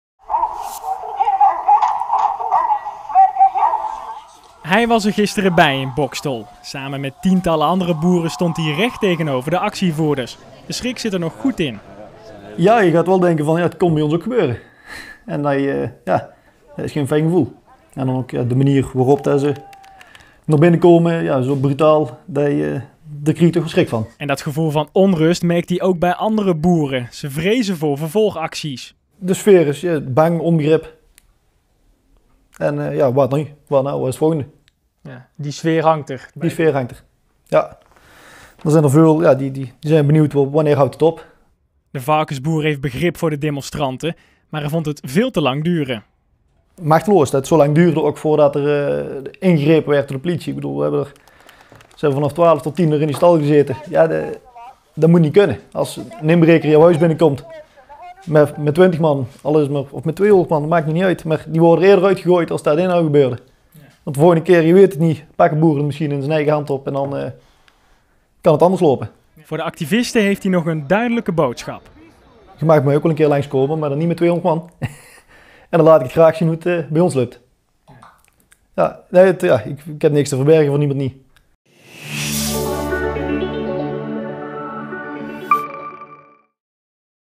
Van boer Willem mogen de dierenactivisten best langskomen, als ze maar begrip hebben voor andersd...
Varkenshouder Willem Berkers (26) uit Vlierden moet de dag na de stalbezetting in Boxtel nog steeds bijkomen van wat er is gebeurd. Hij stond maandag samen met ruim honderd andere boeren aan de achterkant van de stal, met de demonstrerende dierenactivisten in het vizier. “Het zou hier ook kunnen gebeuren. Dat baart me zorgen.”